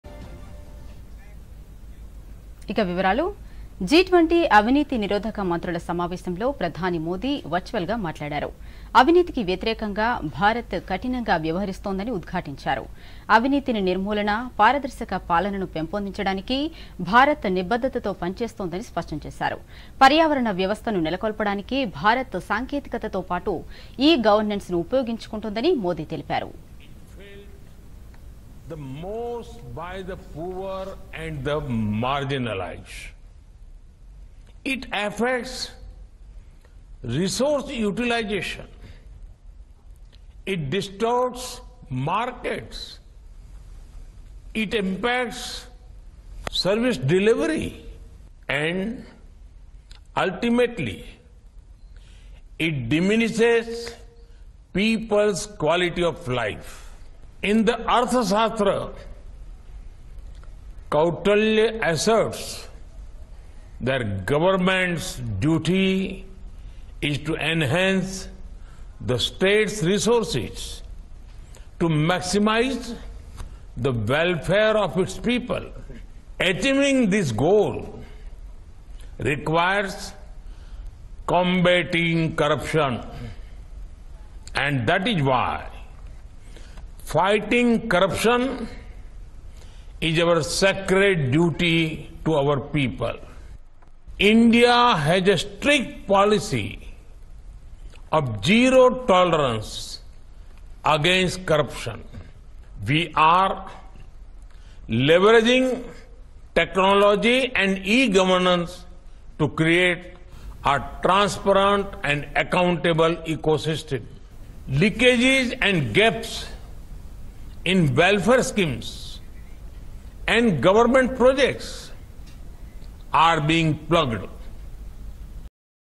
PM Modi addresses G20 anti-corruption ministers meeting | 🟥 DD News Telangana
జీ20 అవినీతి నిరోధక మంత్రుల సమావేశంలో ప్రధాని మోదీ వర్చువల్ గా మాట్లాడారు.. అవినీతికి వ్యతిరేకంగా భారత్ కఠినంగా వ్యవహరిస్తోందని ఉద్ఘాటించారు. అవినీతిని నిర్మూలన, పారదర్శక పాలనను పెంపొందించడానికి భారత్  నిబద్ధతతో పని చేస్తోందని స్పష్టం చేశారు. పర్యావరణ వ్యవస్థను నెలకొల్పడానికి భారత్ సాంకేతికతతో పాటు ఇ-గవర్నెన్స్‌ను ఉపయోగించుకుంటోందని  మోదీ తెలిపారు.